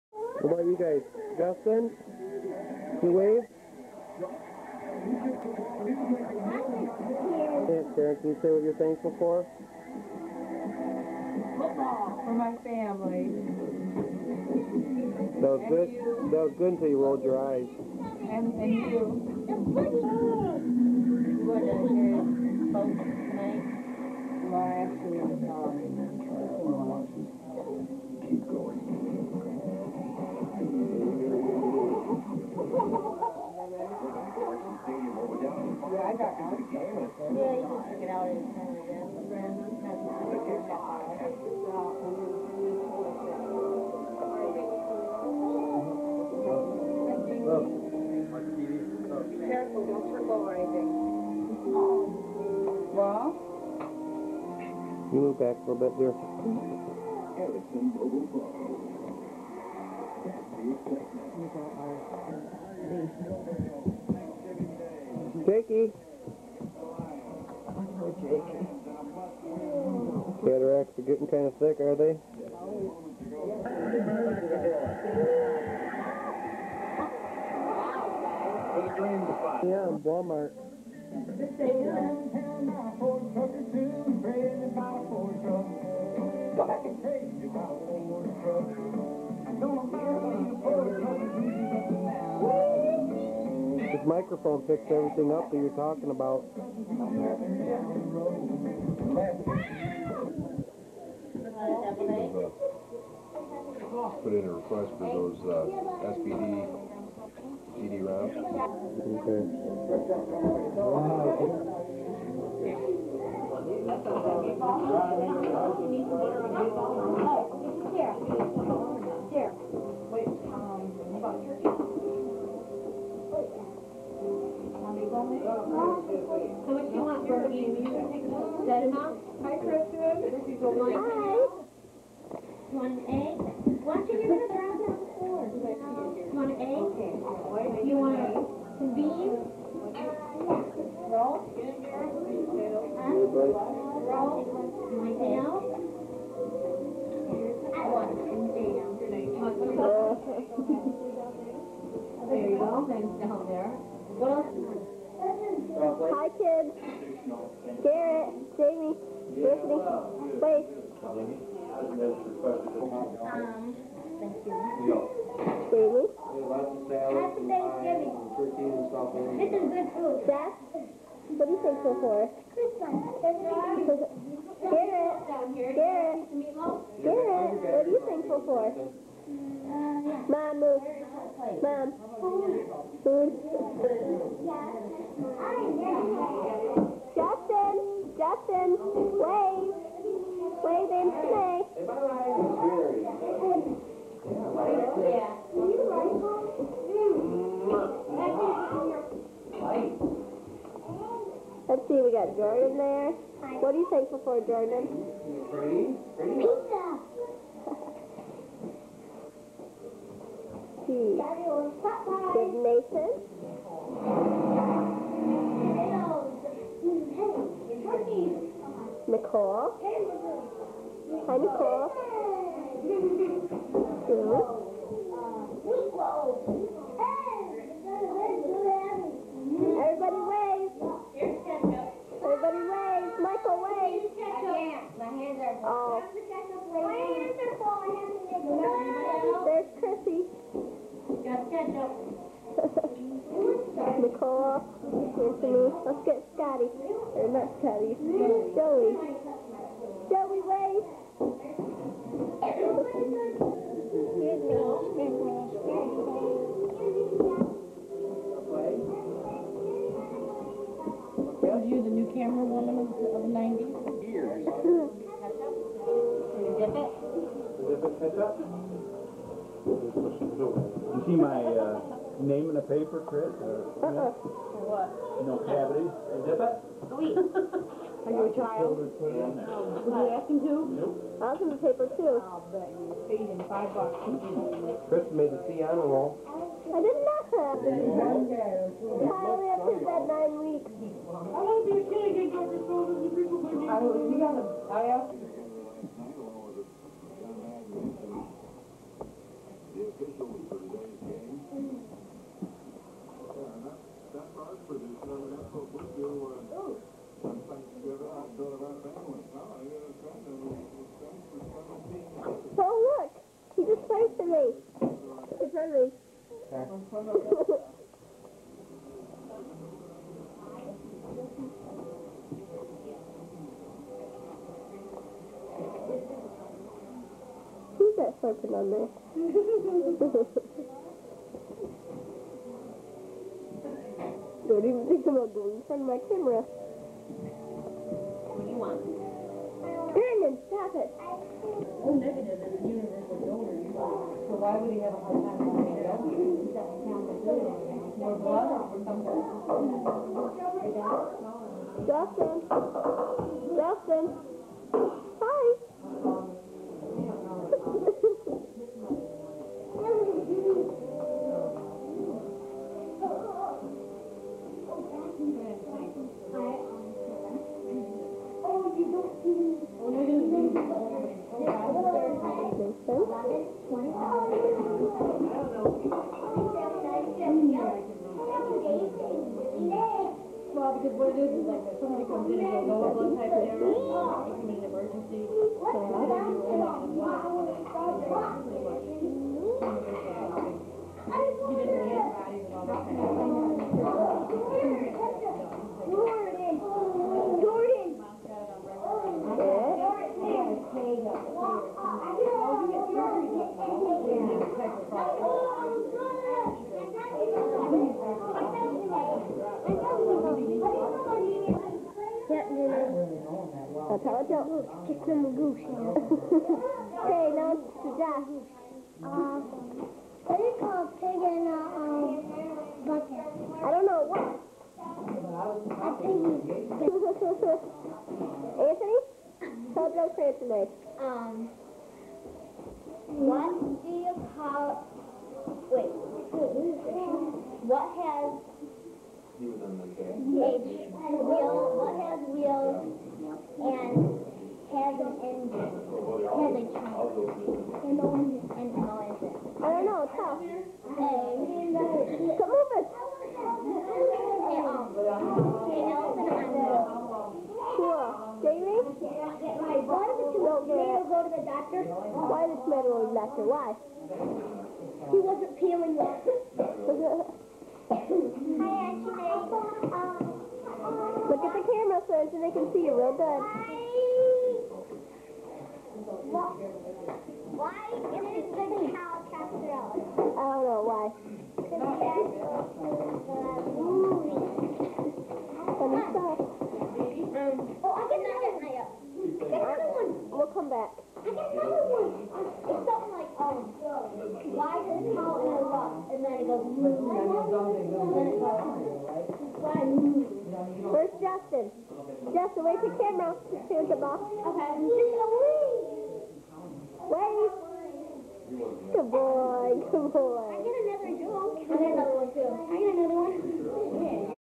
Thanksgiving '98
Thanksgiving party at the Thornton Family Farm.